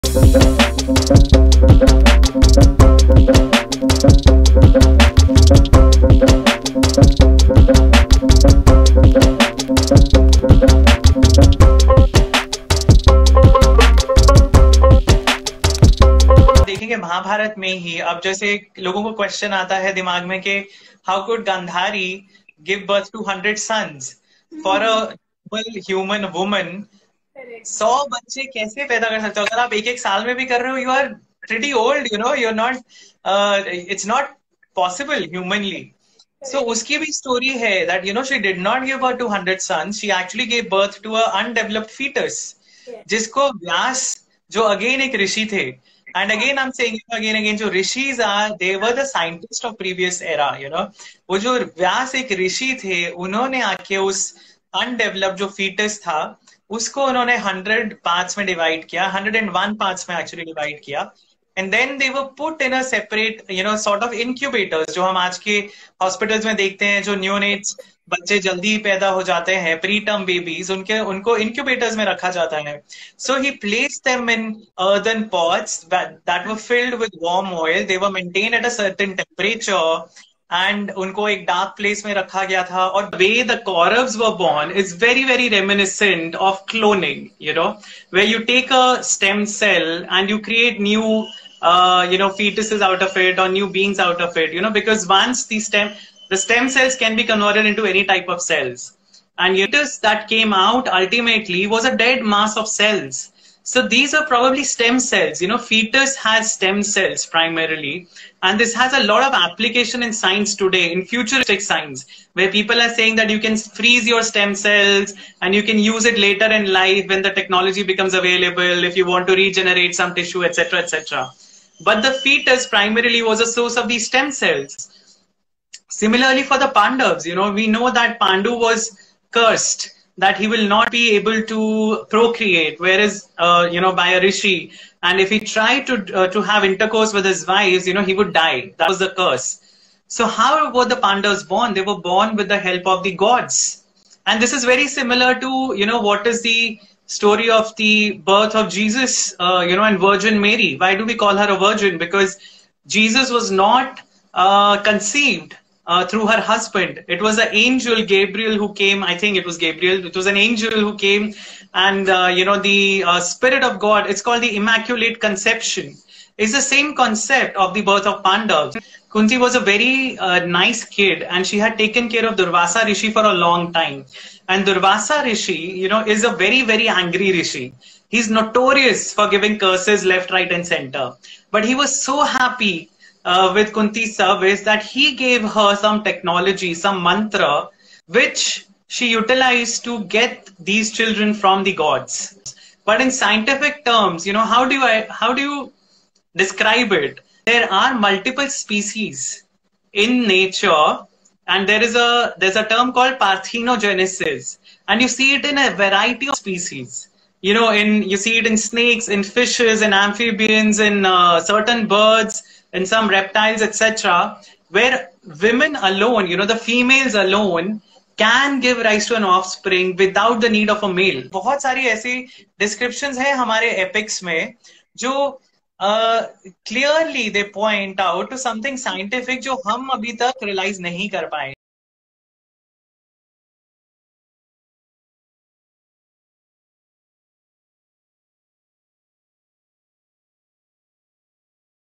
Science behind birth of Kauravs & Pandavs | Dr. Vineet Aggarwal
Many people may have heard stories from the Mahabharat regarding the Kauravs and Pandavs but did you know about their unusual births? Could these stories point toward some scientific phenomena?

See full conversation with  @Dr. Anamika Chawhan

Check the work of Dr. Vineet Aggarwal:-